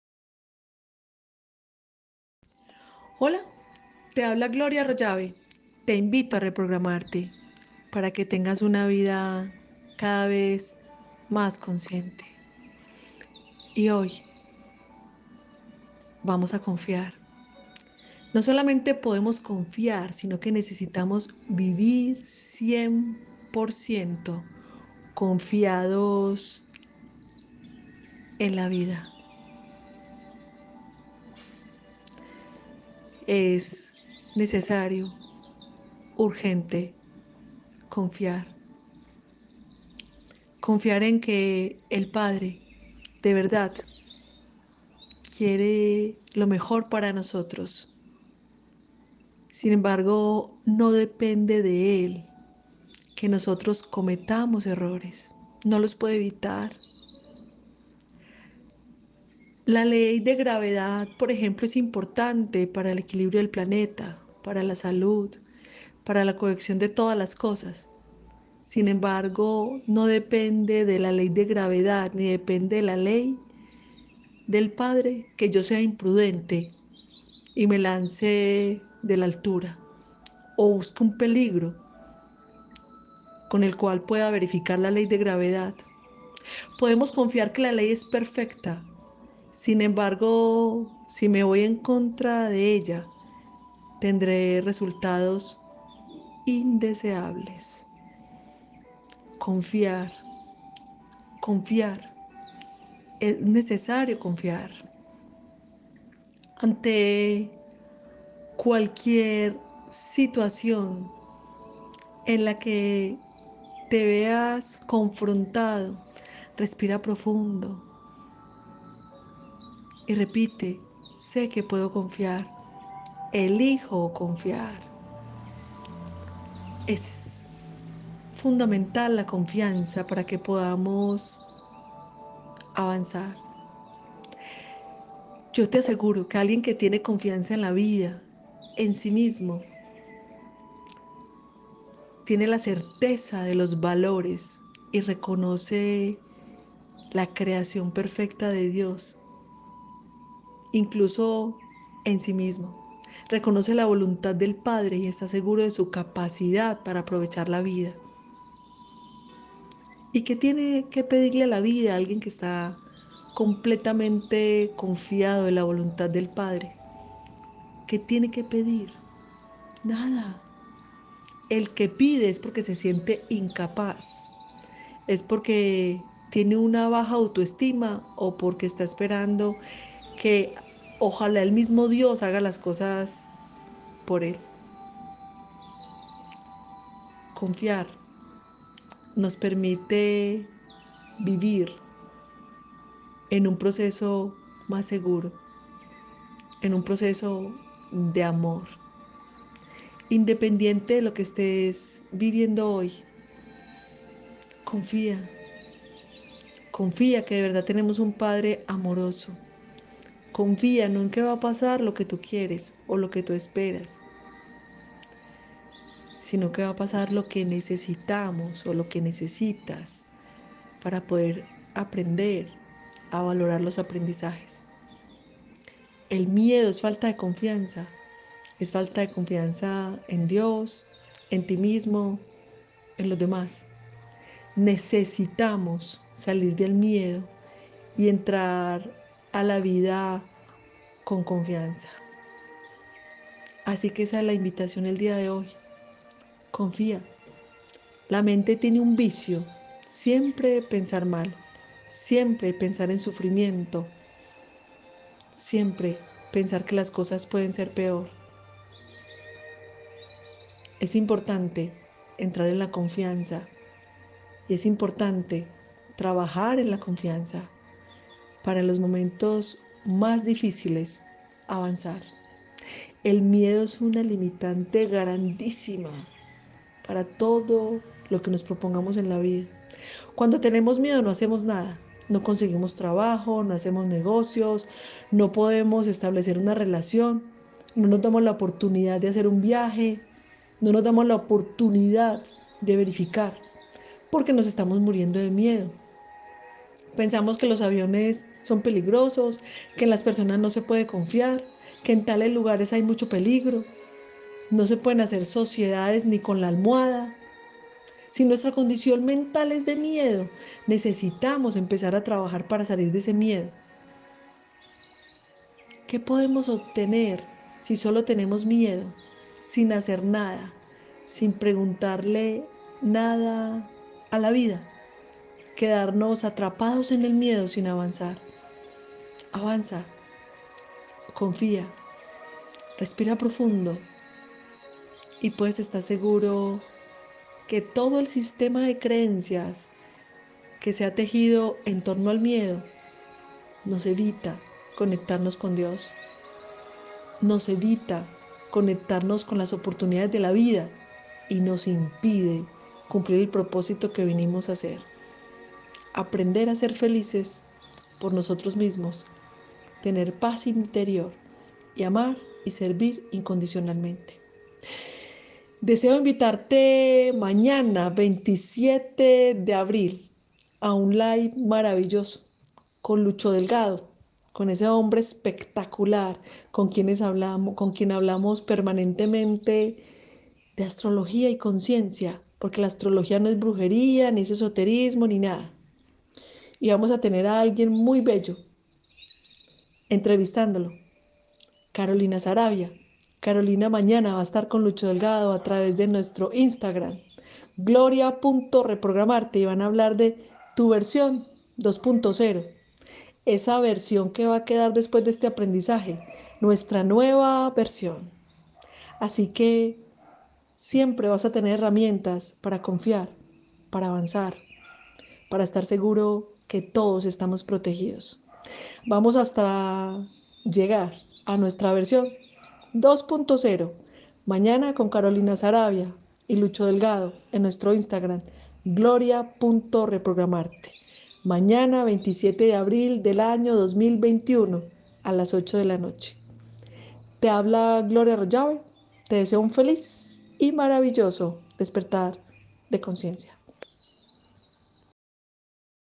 No solamente podemos confiar sino que necesitamos vivir 100% confiados en la vida | Gloria Arroyave
Recuerda que la clave es vivir desde el corazón.

Para recibir las reflexiones diarias, únete a mi canal de Telegram